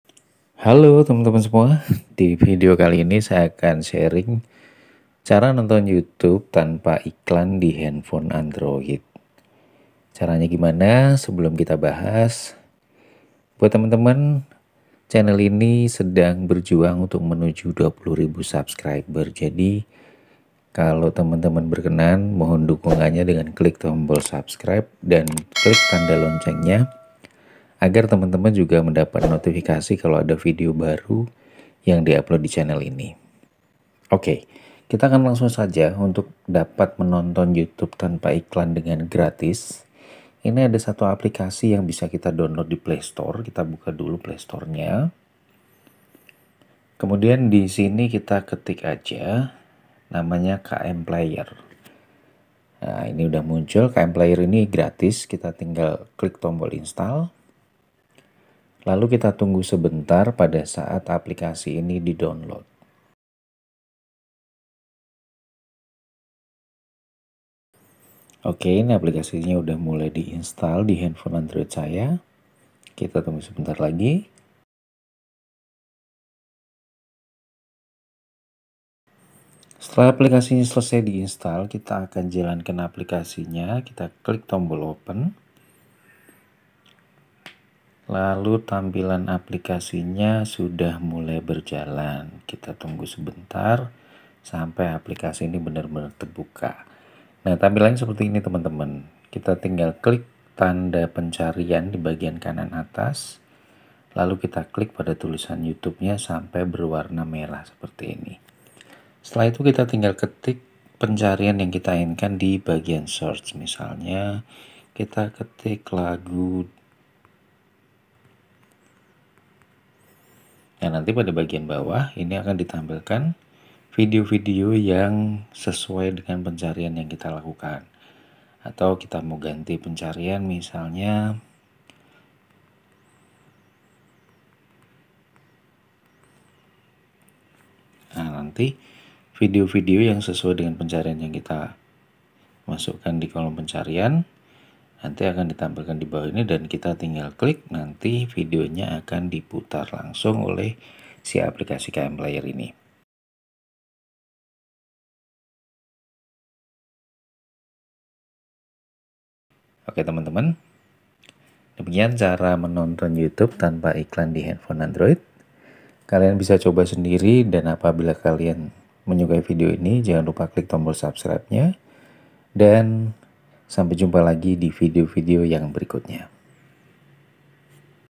Cara nonton youtube tanpa iklan di hp android gratis!
halo...halo...di video kali ini saya coba sharing cara baru nonton youtube di hp android tanpa ada iklan yang muncul. Semoga ini bermanfaat buat kalian yang suka dengerin playlist lagu-lagu atau video lainnya di youtube ya.